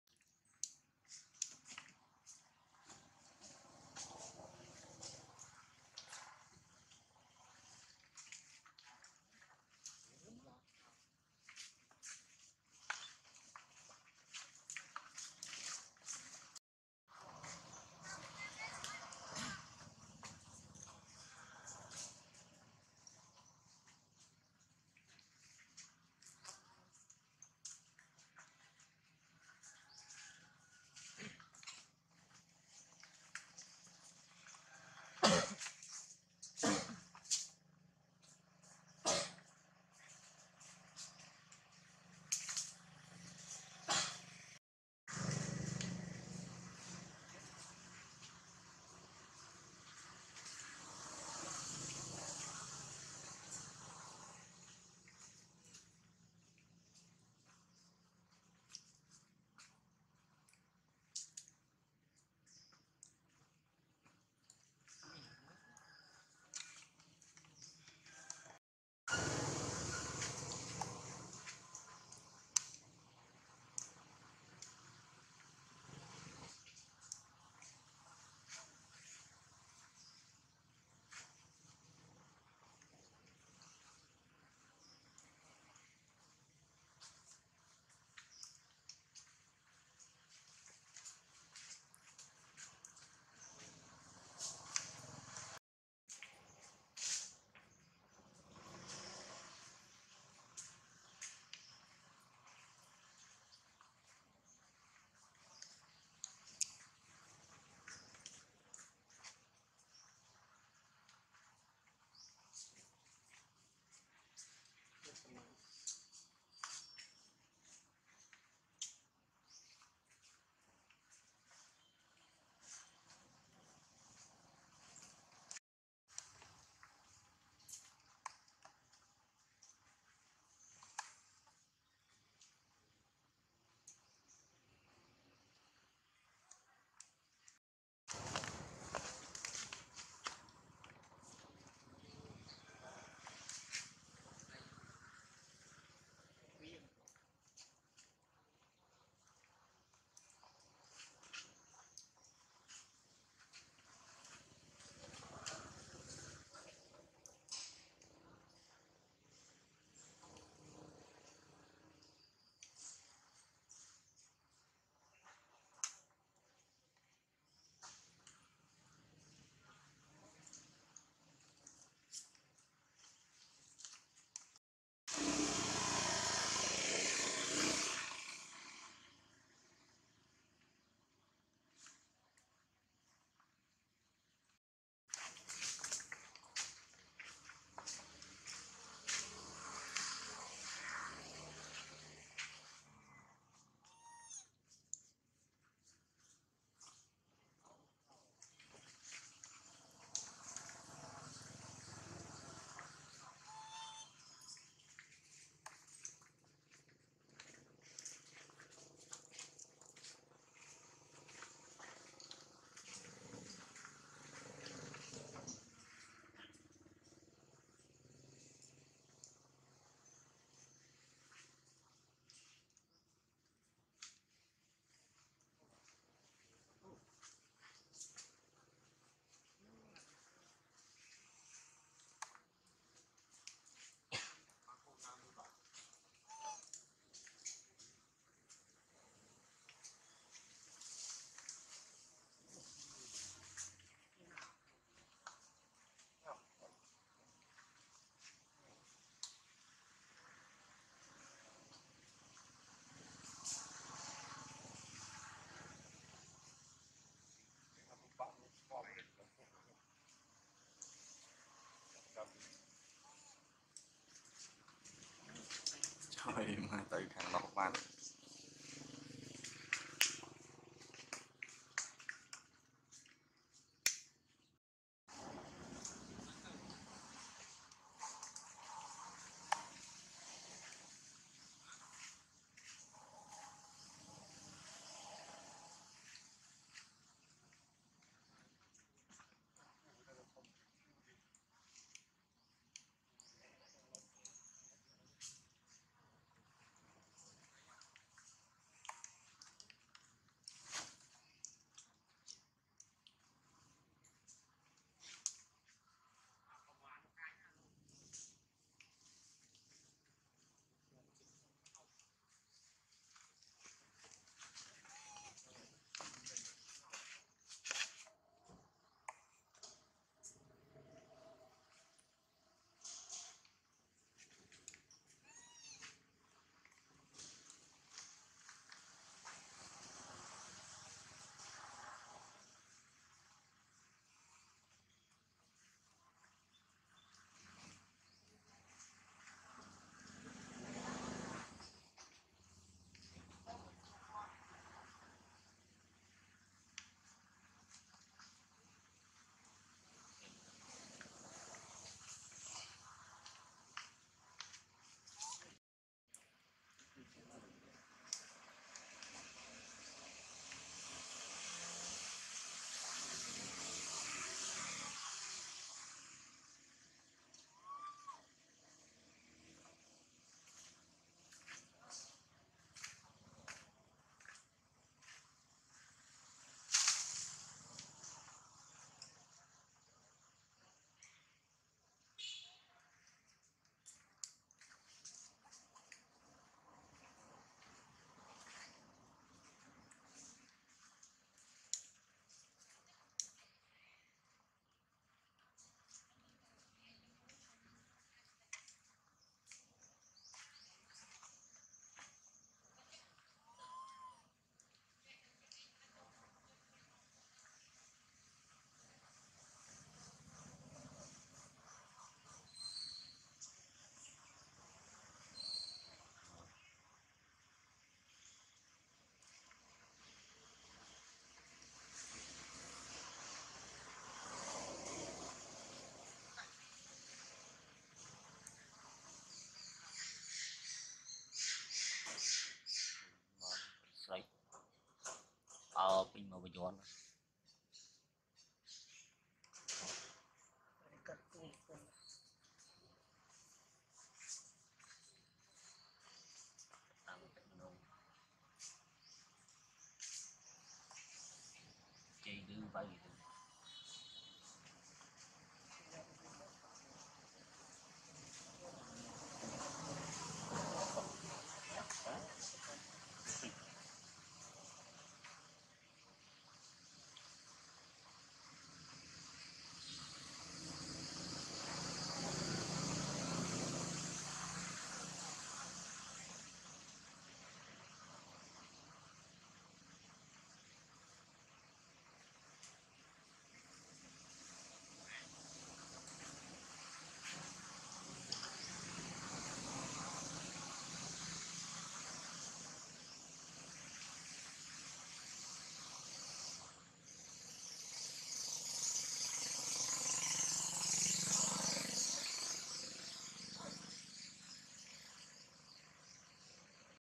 MILLION CARE ! Mom Jade pull baby Jinx for hug and Tell Jinx don't move close to kidnapper it danger
Everyday Monkey